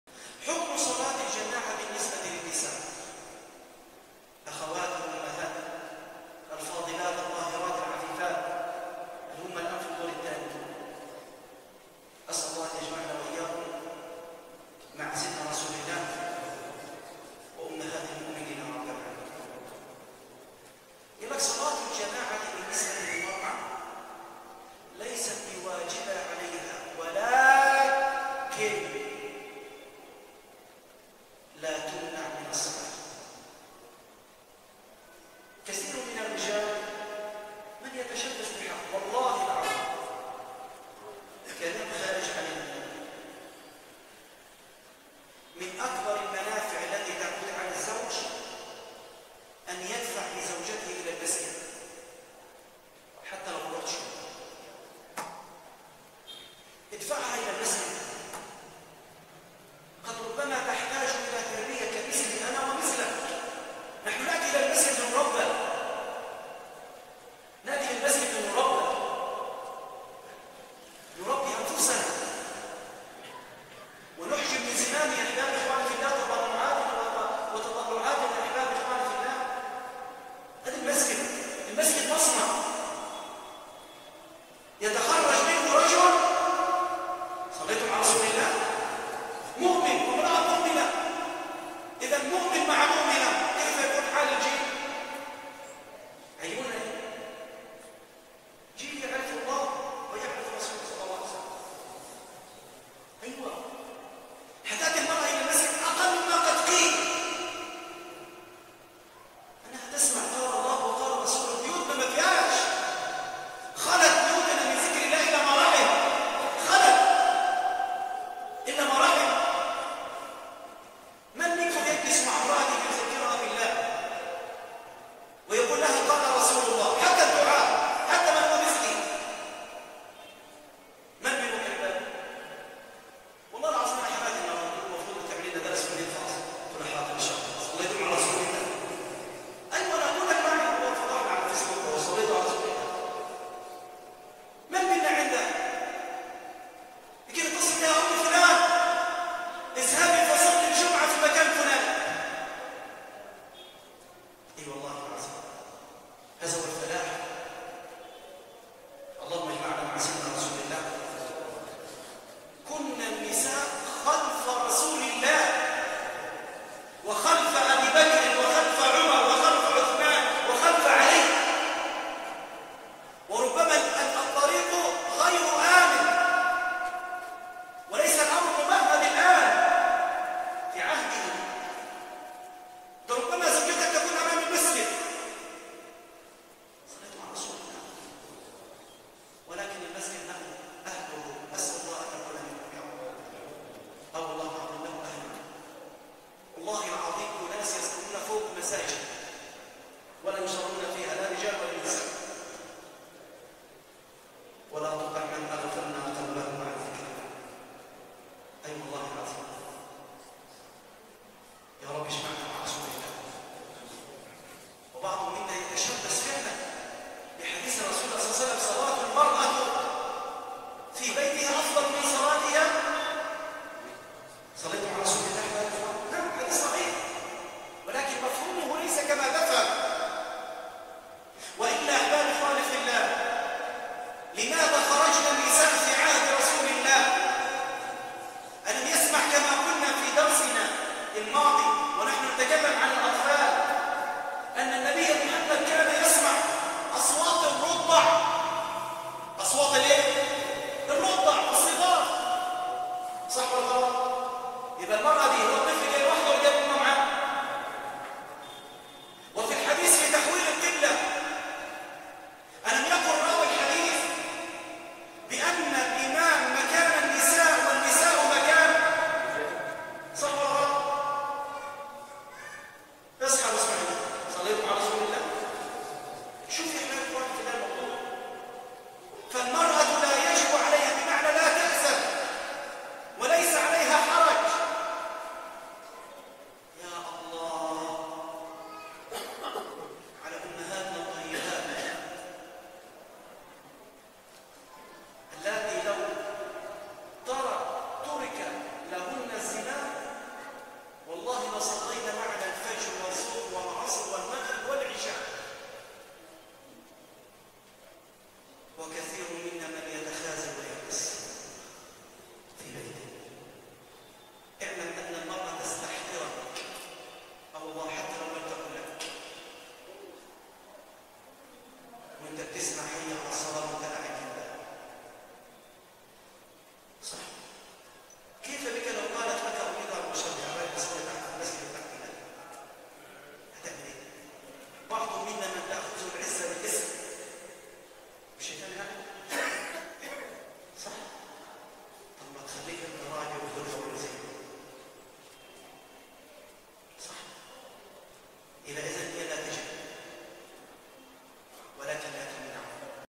حكم صلاة الجماعة للنساء بالمسجد ـ فضيلة الشيخ طلعت على الدومى
اقل دعم ممكن توصله لينا هوا اشتراك بيالقناه ولايك للفيديو فضلا وليس امرا
ولا تنسونا من صالح دعائكم
وجزاكم الله خيرا ..